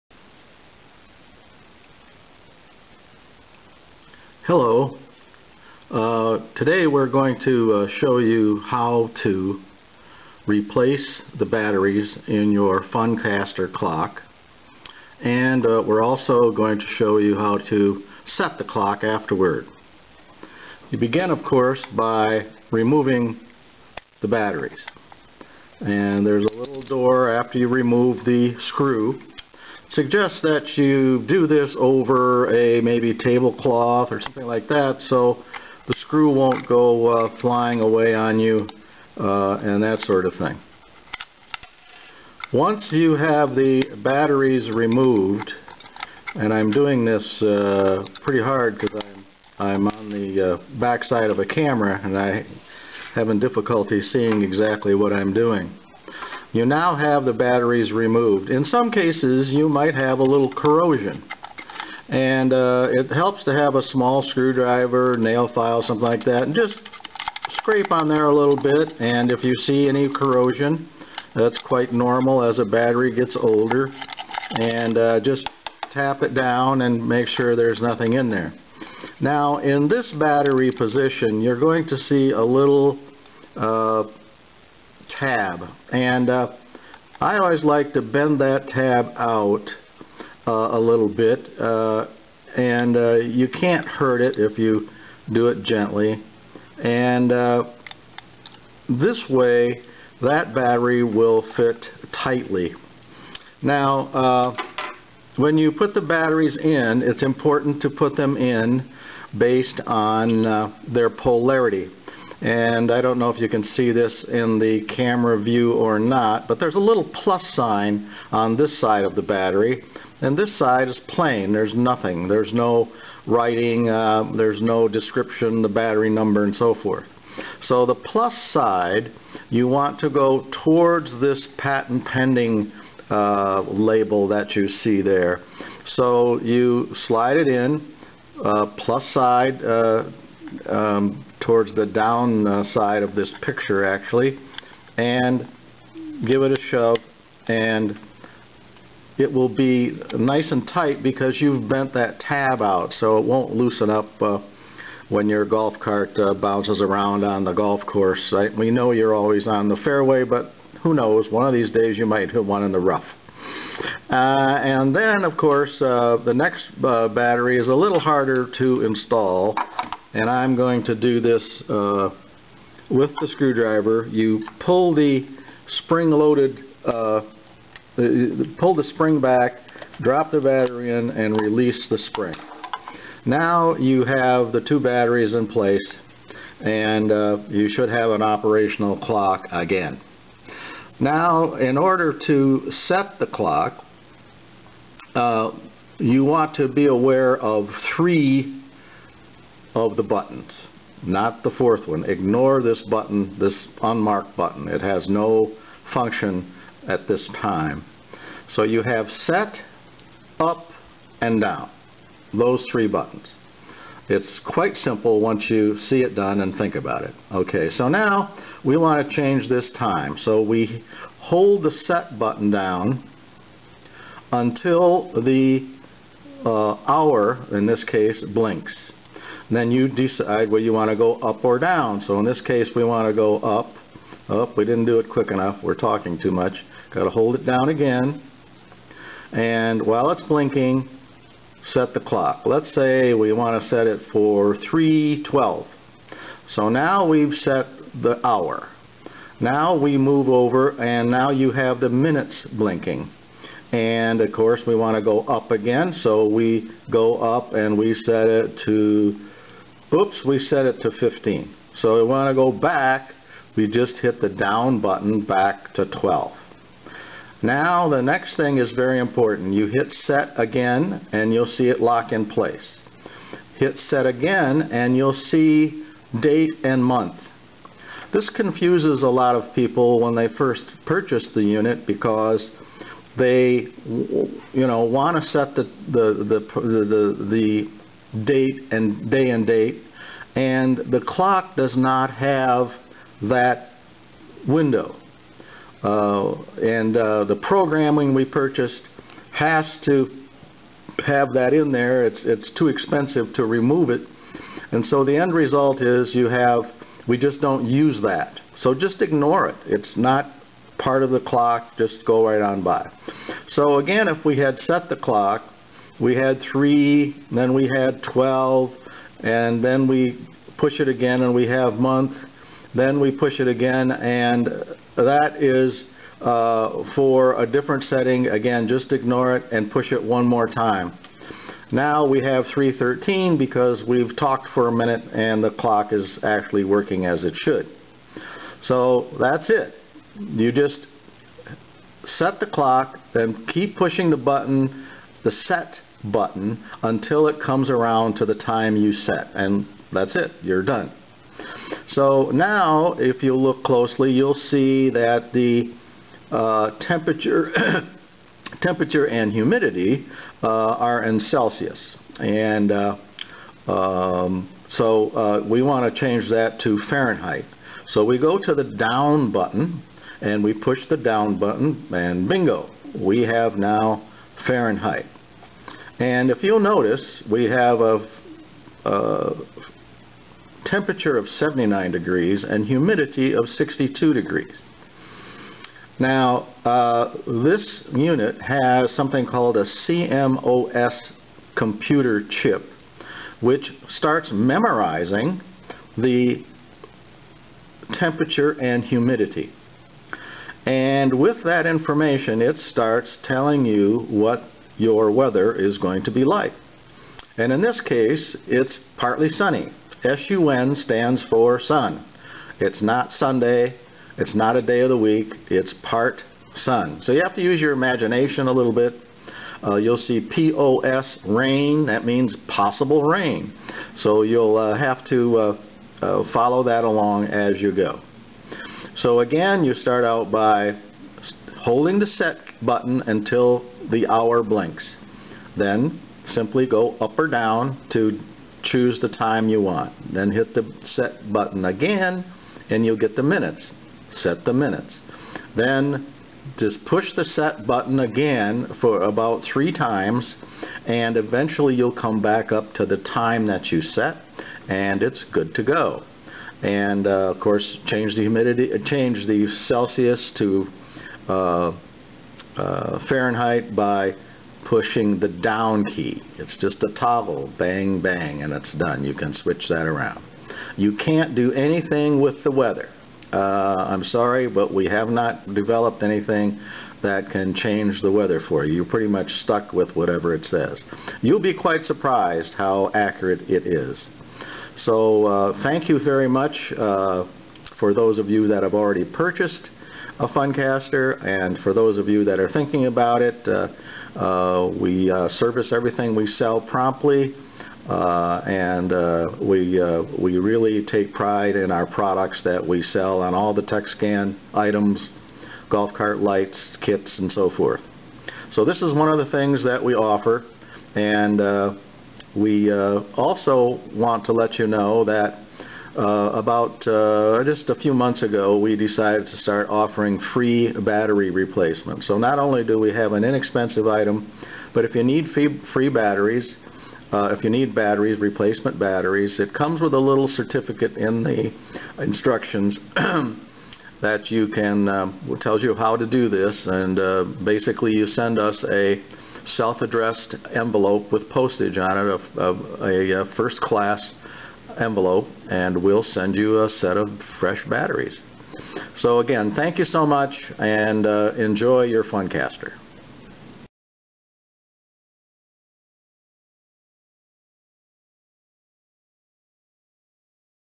FUNcaster Battery Change and Reset
How to the change the batteries and reset a FUNcaster weather clock